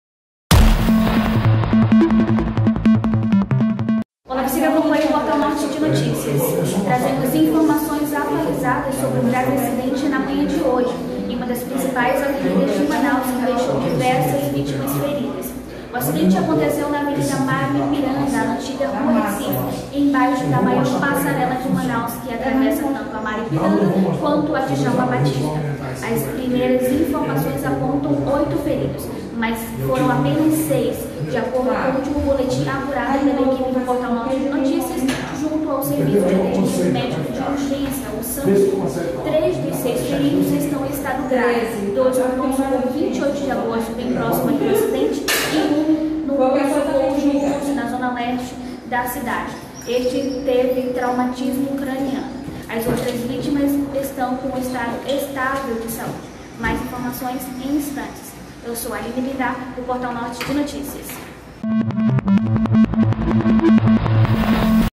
ATUALIZAÇÃO SOBRE ACIDENTE COM VITIMAS NA MANHÃ DE QUINTA,13
Alinne Bindá atualiza informações sobre o atropelamento coletivo no inicio desta manhã